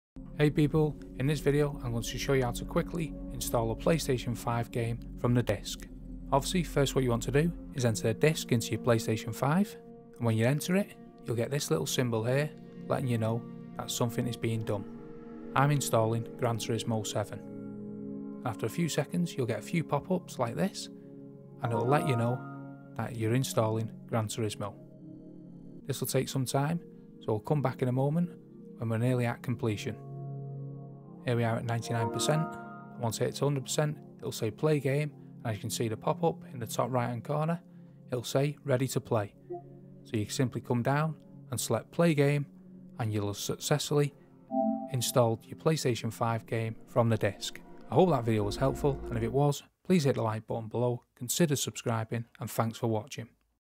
How to Install a PS5 Game From Disk
In todays video we are going to learn How to Install a PS5 Game From Disk.